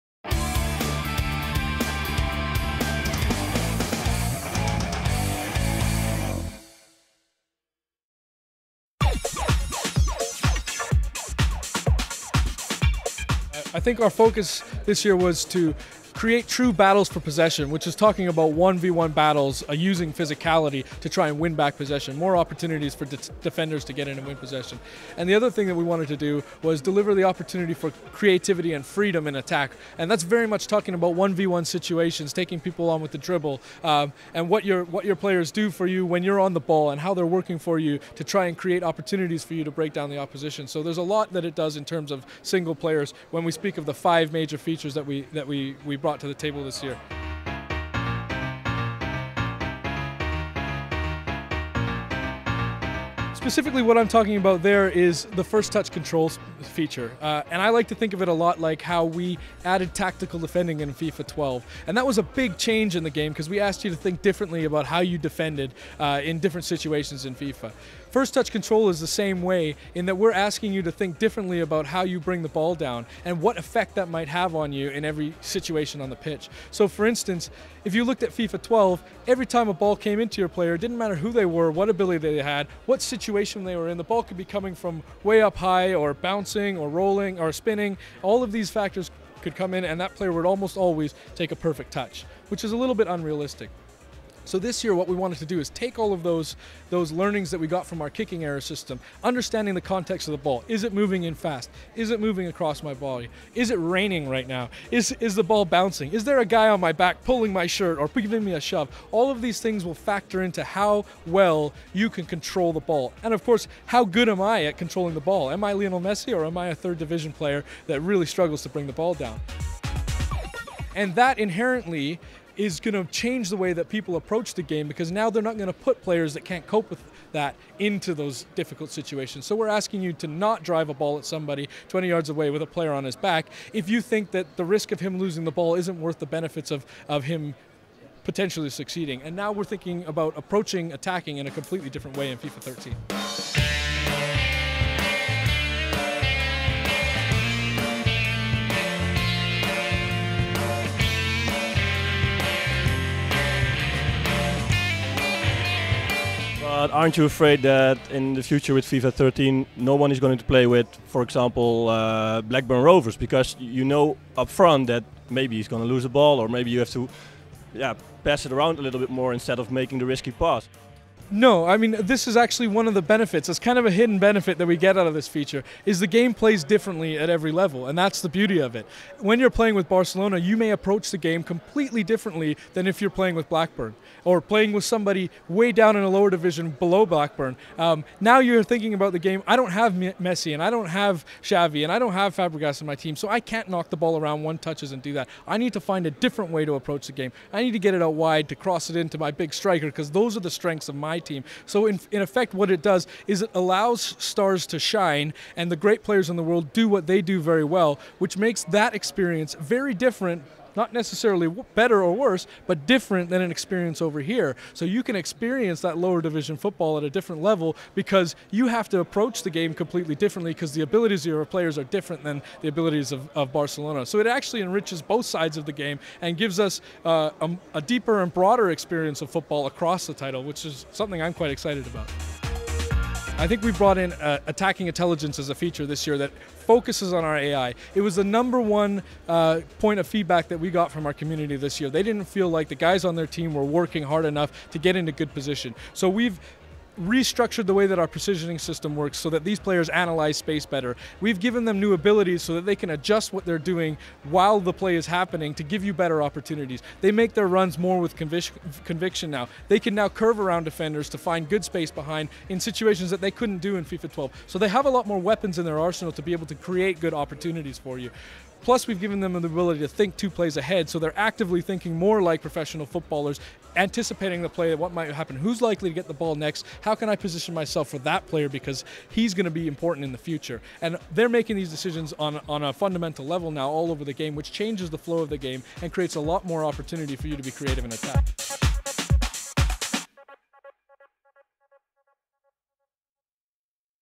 Interview FIFA 13
Gamer.nl sprak met de maker van FIFA 13 over de vernieuwingen van dit jaar!